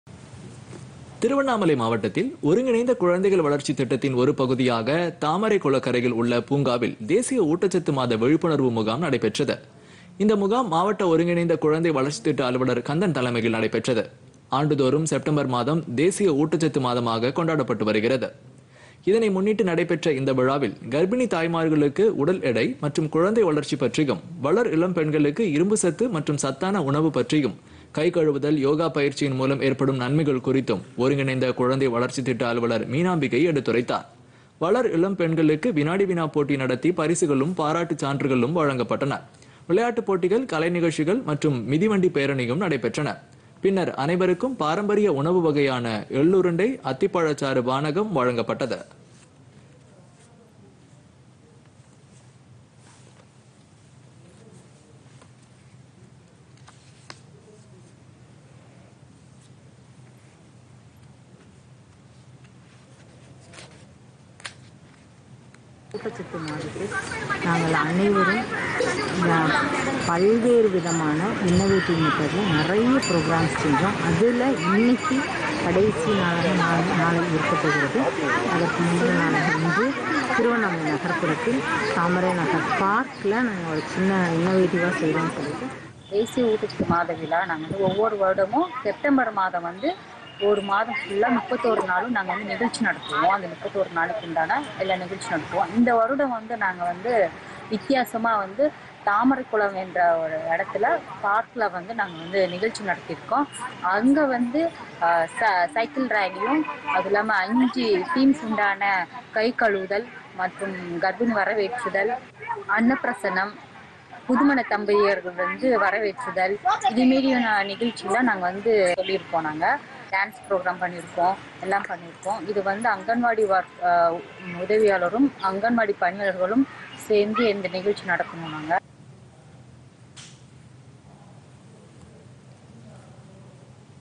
PodhigaiTamilNews பொதிகைசெய்திகள்
திருவண்ணாமலை மாவட்டத்தில் ஒருங்கிணைந்த குழந்தைகள் வளர்ச்சி திட்டத்தின் ஒரு பகுதியாக  தாமரைக் குளக்கரையில் உள்ள பூங்காவில் தேசிய ஊட்டச்சத்து மாத விழிப்புணர்வு முகாம்  நடைபெற்றது.   ​